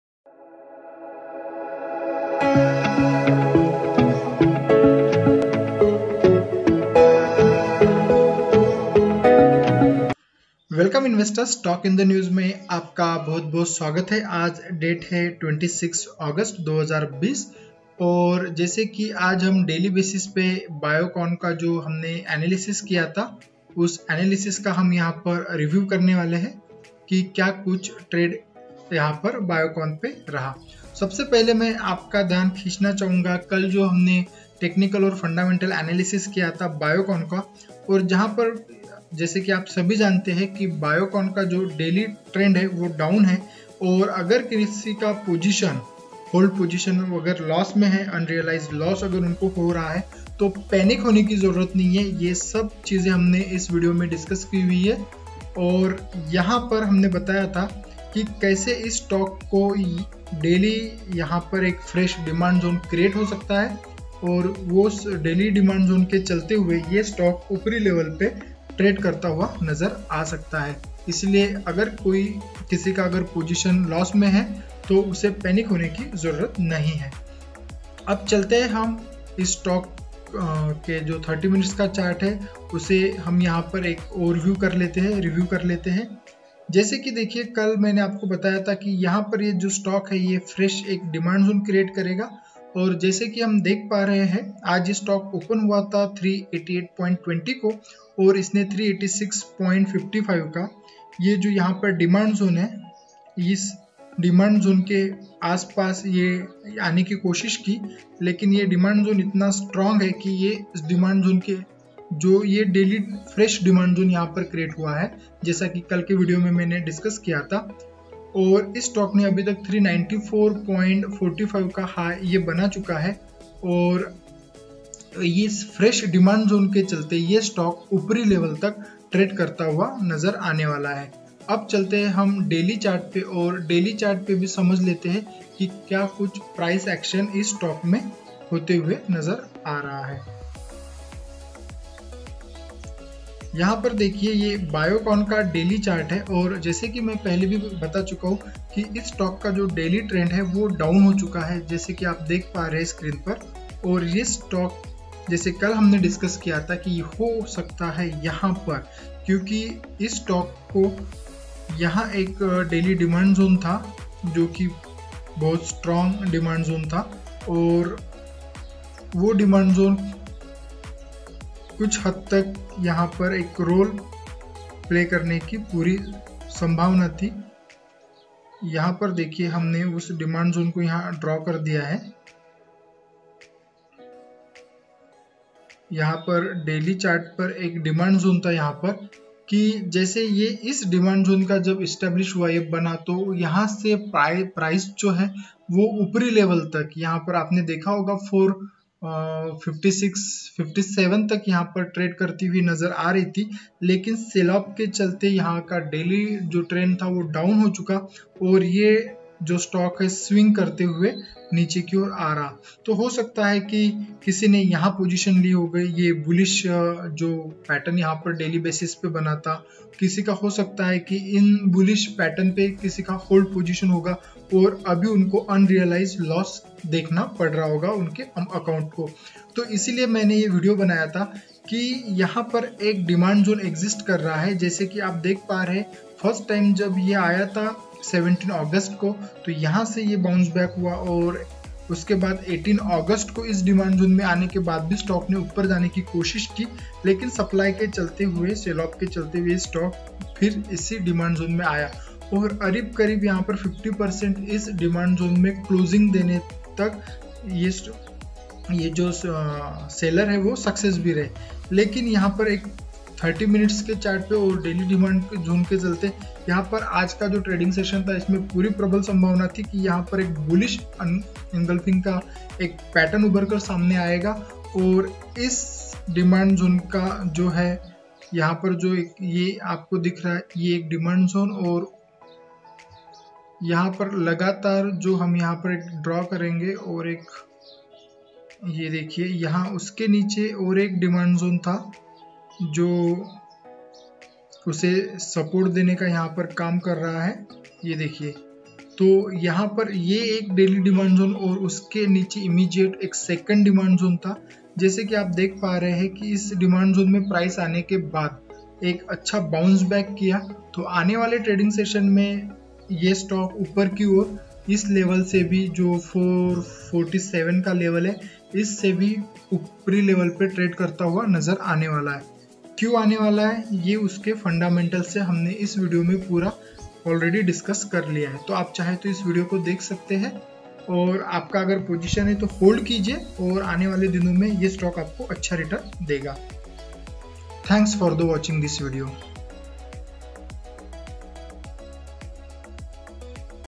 Biocon Stock Fundamental and Technical Analysis Reviews
This video is to learn you how to find out demand and supply zone. Biocon Stock Fundamental and Technical Analysis Reviews. Demand and supply is a very big concept and this concept is also working in the stock market.